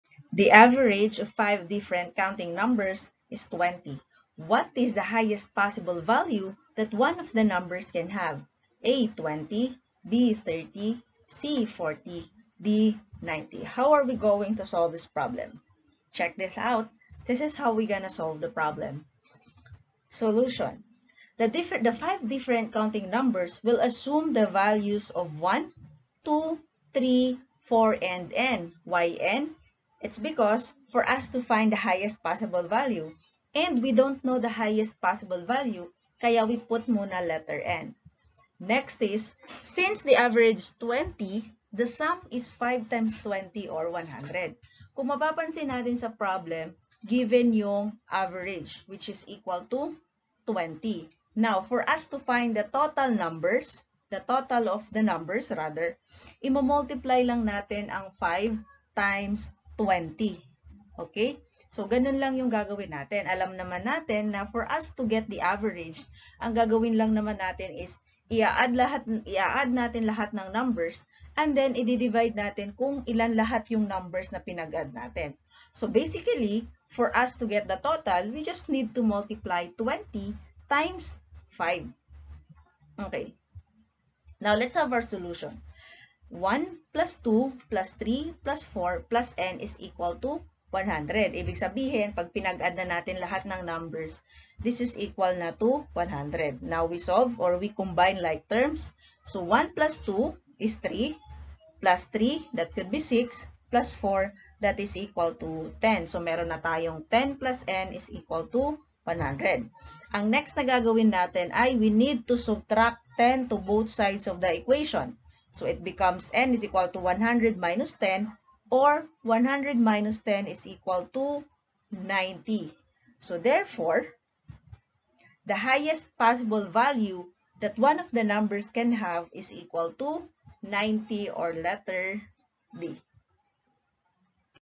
REVIEW TO PASS Part 3: LET and Civil Service Exam Math Review with Solutions and Explanations
This video is created to help you prepare for the Exam.